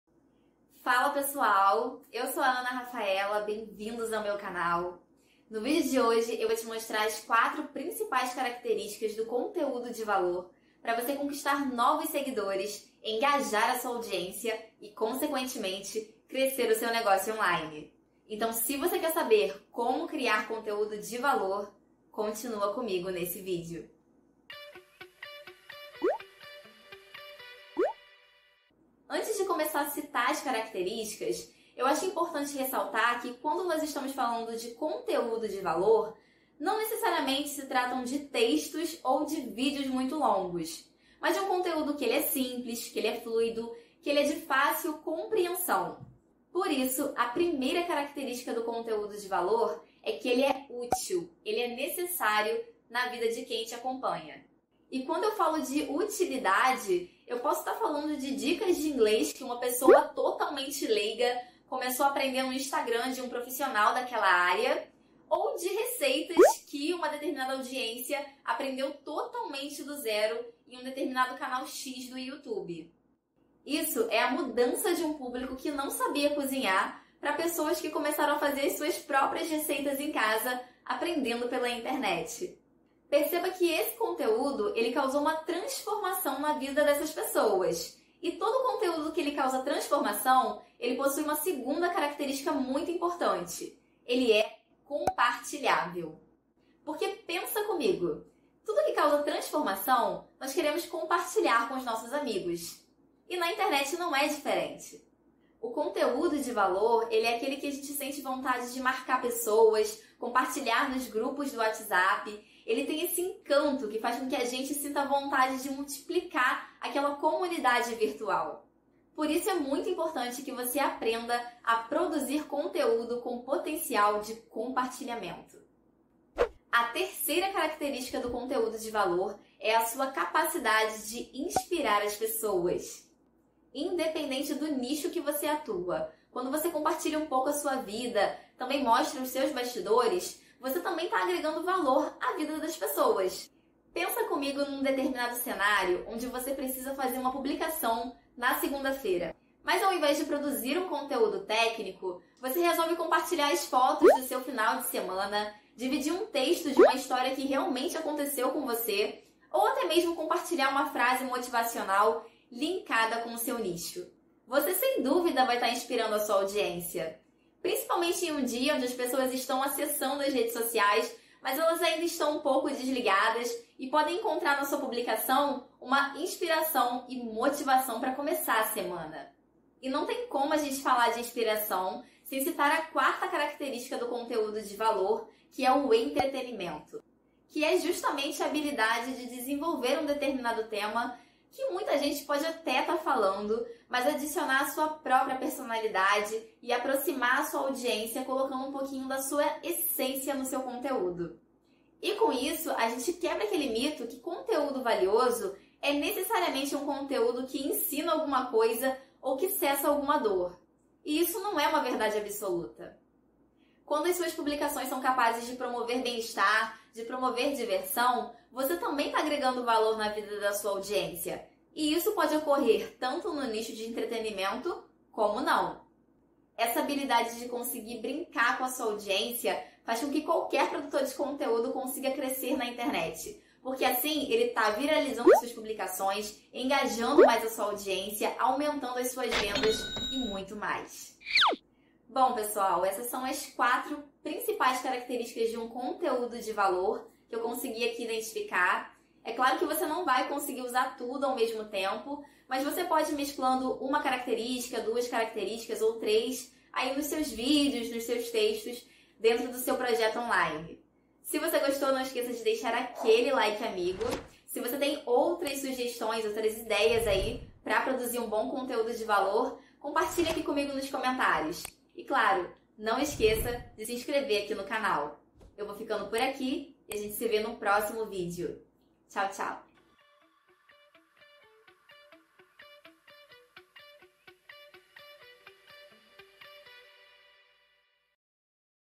Como Criar Conteúdo de Valor | Ana Rafaella
Afinal ... Como Criar Conteúdo de Valor ? Olá meus amores! Nesse vídeo eu decidi compartilhar com vocês um Passo a Passo muito simples e muito pratico que eu utilizo pra construir conteúdo de valor e fortalecer a minha marca pessoal .. Espero que gostem !

Abaixo, informações que podem te ajudar 😉 !

💙 Aprenda a criar você mesmo o seu Negocio Online, conheça o curso Formula Negócio